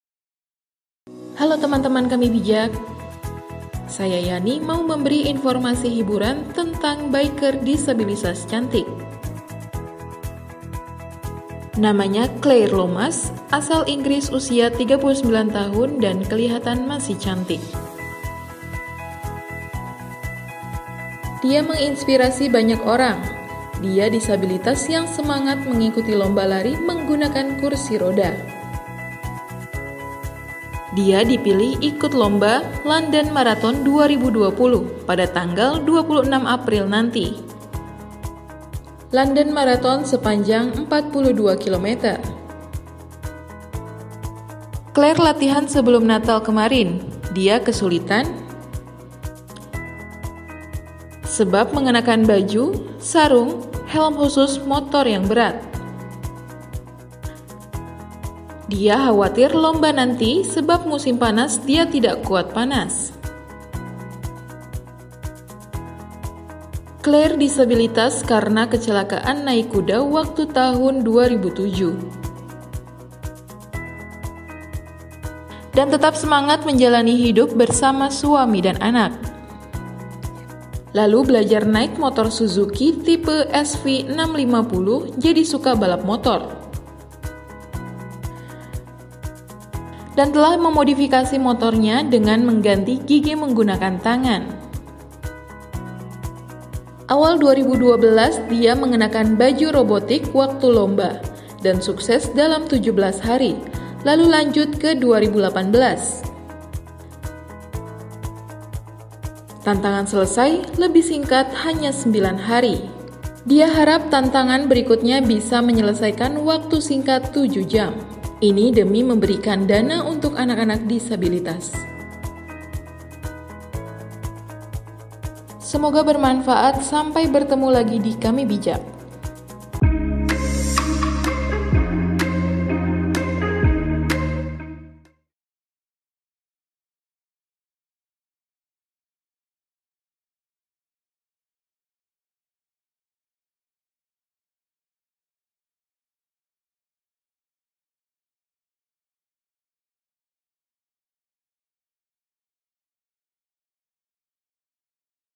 Demi Anak-anak Disabilitas, Biker Difabel Cantik Ikuti Marathon Sejauh 42 Km
Hiburan, kamibijak.com – Seorang perempuan penyandang disabilitas di Inggris mengikuti lomba marathon dengan rute sejauh 42 kilometer. Aksi yang ditujukan dalam rangka penggalangan dana untuk anak-anak difabel ini menuai decak kagum.

Penyandang disabilitas cantik berusia 39 tahun itu adalah Claire Lomas. Dia adalah biker difabel yang menginspirasi banyak orang berkat semangatnya dalam melakukan aksi sosial meskipun hidup dengan keterbatasan. Keikutsertaan Claire dalam lomba marathon membuat geger dunia maya karena dinilai menantang kelemahan dirinya sendiri.

Claire akan mengikuti kompetisi maraton menggunakan kursi roda. Kompetisi yang dipilihnya adalah London Marathon 2020, diselenggarakan 26 April 2020 mendatang.

Sekedar informasi, London Marathon merupakan sebuah kompetisi dengan rute mencapai 42 kilometer. Dalam melakukan aksinya, Claire tidak hanya menggunakan kursi roda. Dia akan mengenakan riding gear lengkap dari wearpack Spidi sampai helm Shoei X-Spirit III atau X-Fourteen.

Dikutip dari MCN, Claire telah berlatih dan mempersiapkan diri sejak sebelum natal. Ia sempat kesulitan saat latihan mengenakan riding gear komplit.

Baginya, wearpack kulit yang harus digunakan sangat mengekang dan membuatnya khawatir saat melakukan marathon nanti. Apalagi cuaca musim panas telah dimulai sejak bulan januari, hal ini berdampak pada tubuhnya yang akan kesulitan beradaptasi di suhu tinggi.

Claire mengalami disabilitas akibat kecelakaan naik kuda di tahun 2007. Meskipun mengalami disabilitas dia tetap bersemangat demi keluarga dan 2 anaknya. Kini Ia punya hobi baru yaitu riding.

Hobi tersebut mulai Ia geluti setelah memiliki sepeda motor Suzuki SV650. Kini Claire menyukai balapan di sirkuit menggunakan motor andalannya Ducati SuperSport. Motor andalannya ini telah dimodifikasi supaya bisa ganti gigi menggunakan tangan.

Claire yakin dan bertekad untuk bisa menaklukan tantangan London Marathon 2020. Apalagi Ia pernah berulang kali menyelesaikan London Marathon sebelumnya.

Berawal di tahun 2012 di mana Claire menggunakan pakaian robotik dalam kompetisinya dan sukses melakukannya selama 17 hari. Dilanjutkan di tahun 2018 dengan tantangan yang sama dan sukses diselesaikan lebih singkat yaitu 9 hari.

Claire berharap untuk tantangannya kali ini bisa dilakukan dalam waktu 7 jam dengan kursi roda dan riding gear lengkap. Aksinya kali ini ditujukan untuk penggalangan dana Whizz-kid yang ditujukan sebagai bantuan untuk anak-anak penyandang disabilitas.